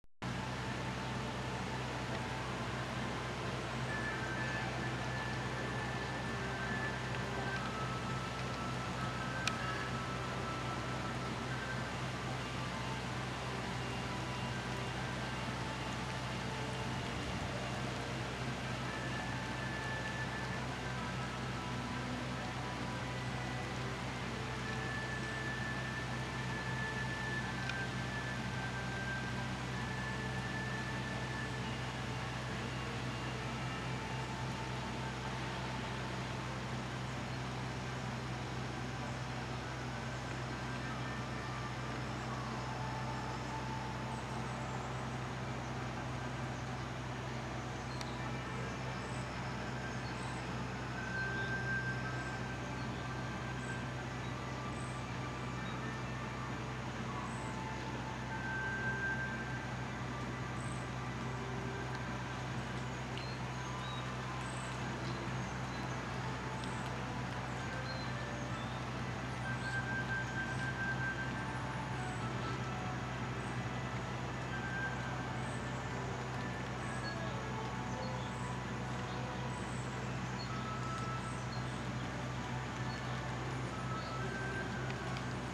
Rainy Day Red Panda
Kiah is looking for any excuse to get away from the kids. But she rather be nagged then get too wet.

Kiah {female} and Rinzen {male}
Columbus Zoo
Columbus, Ohio